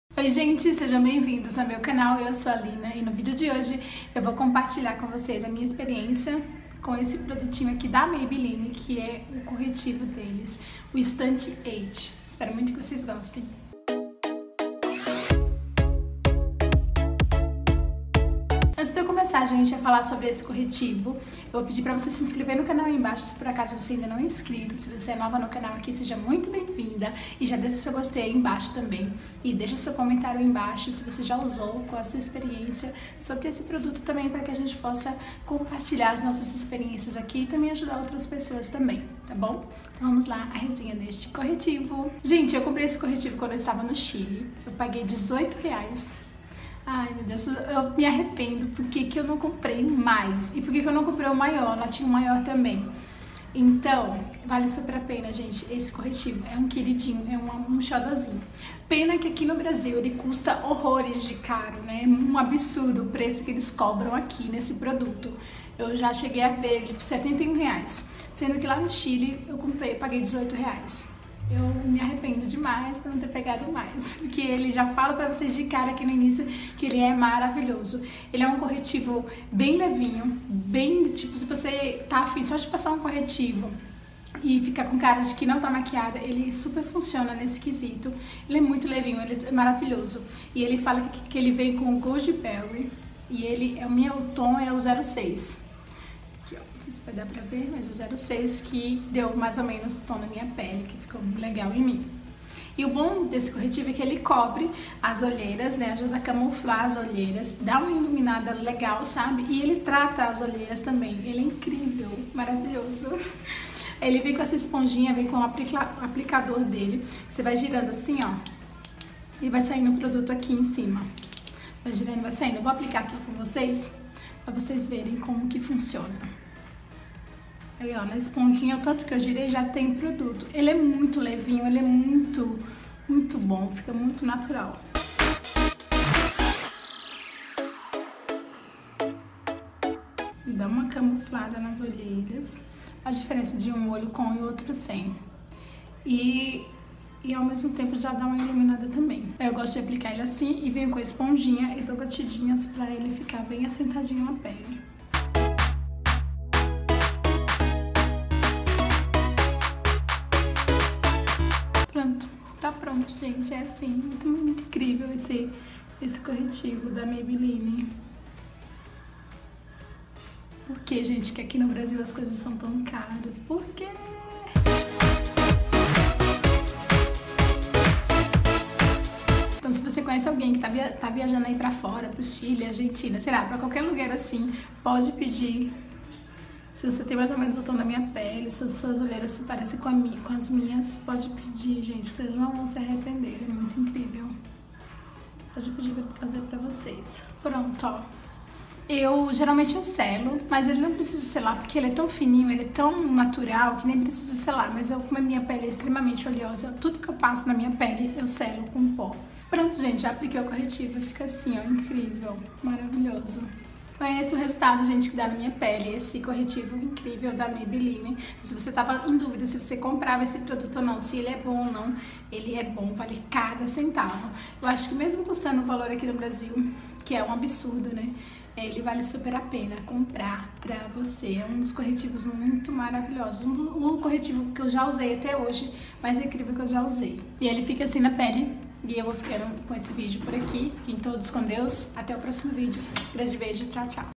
Corretivo Maybelline Instant Anti Age | Makeup By Me | Lina Vaidosa
Oi, gente linda de meu Deus! Tudo bem com vocês? No vídeo de hoje eu compartilhei com vocês a minha experiência com o corretivo da Maybelline. Espero muito que vocês gostem!